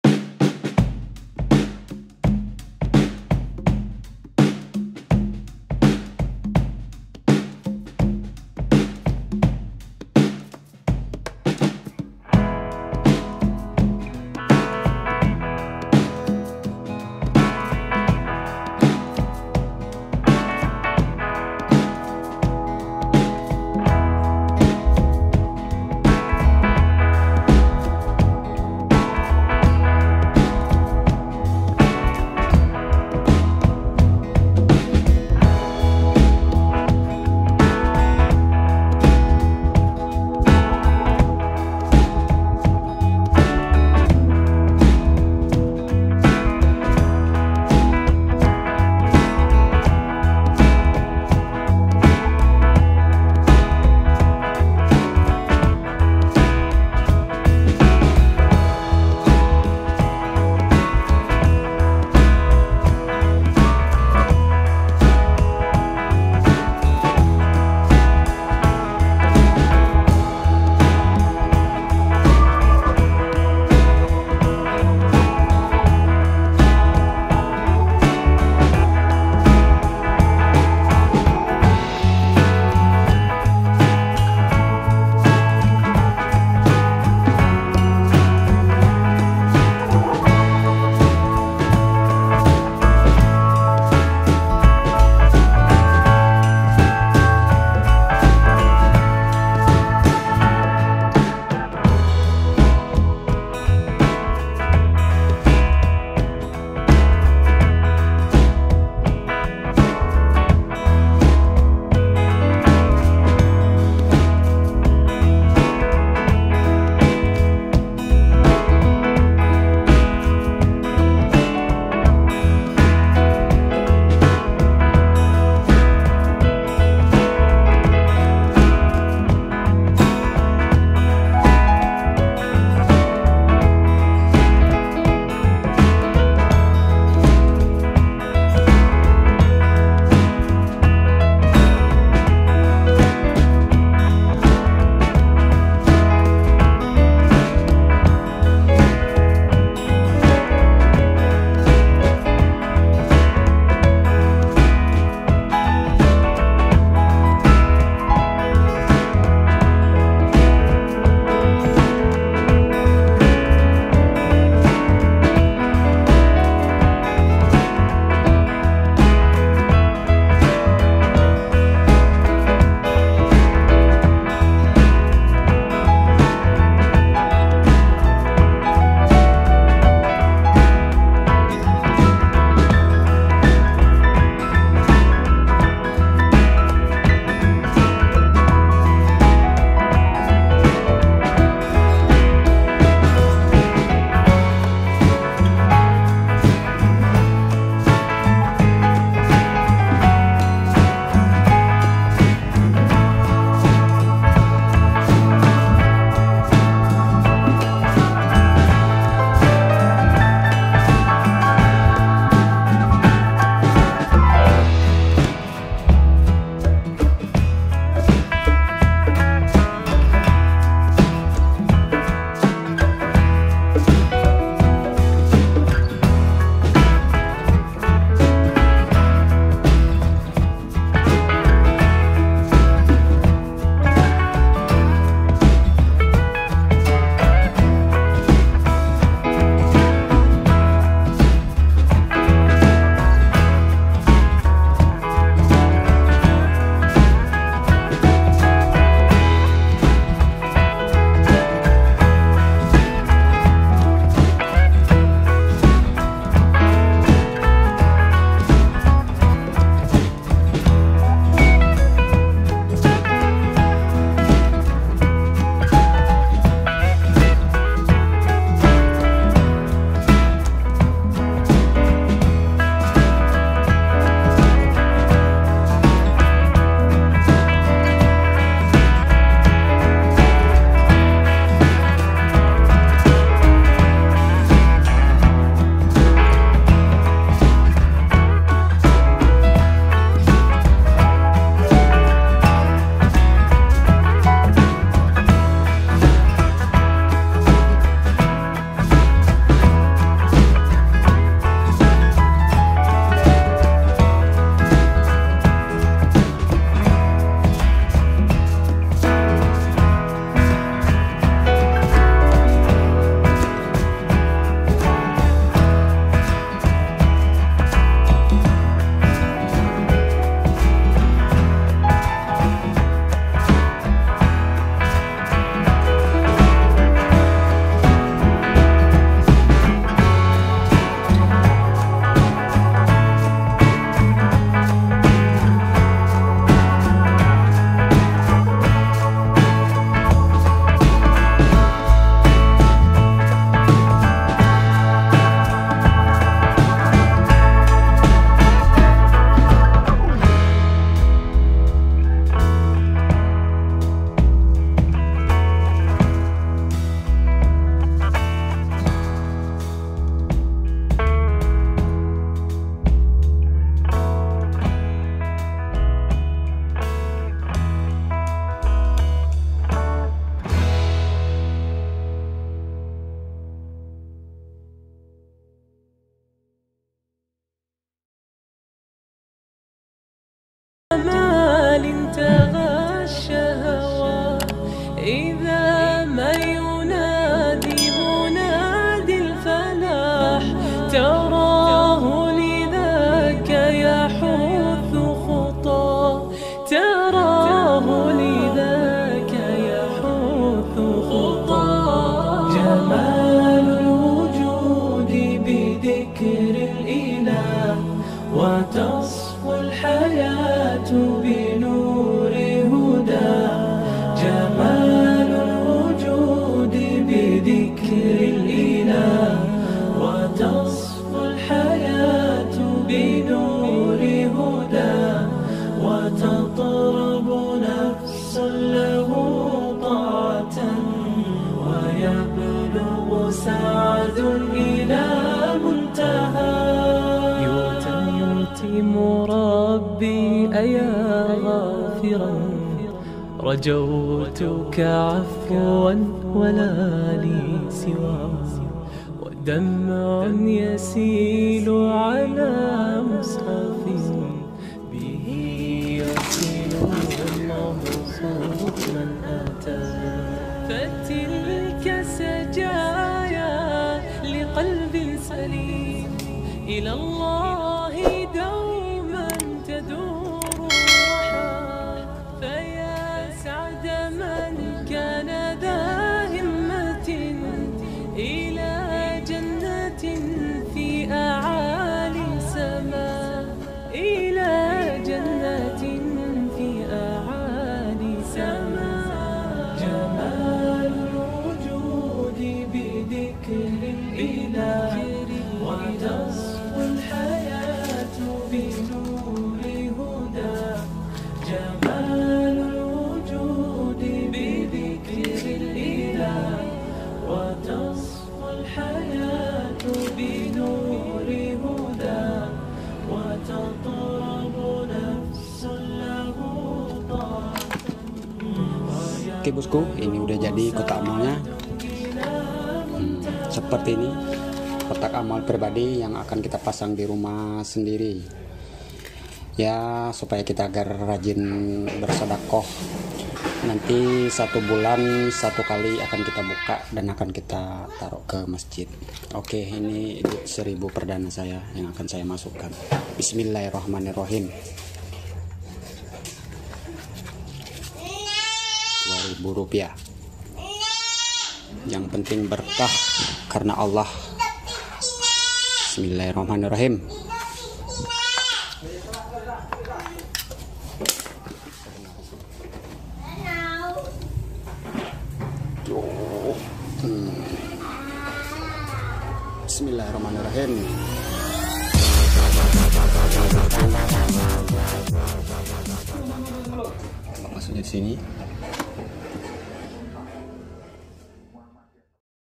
celengan pribadi di rumah| Agar Kita tidak lupa Sedakoh
Assalamualaikum bossQyu kali ini kita akan ngeVlogging memasang atau membuat kotak amal. Mini dirumah tujuan nya agar sering sering sedakoh dan tidak melupakan inpak Ok bos. Semoga bermanfaat target membukanya minimal 1 bulan sekali.kita buka lalu kita sumbangkan. Ke masjid.